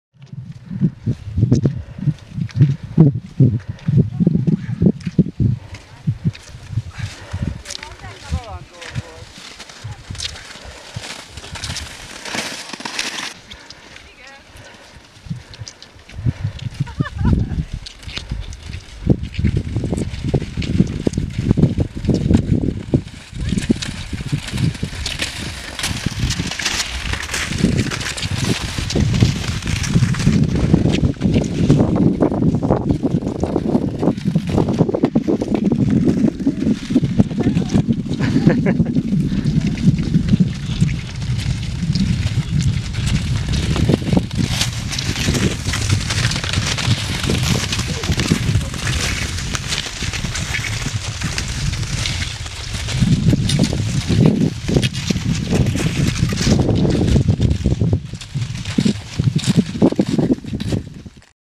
A haladó csapat így kapaszkodik fel a pálya legmagasabb pontjára
A felvétel a hohentauerni táborban készült, a haladó csoportról.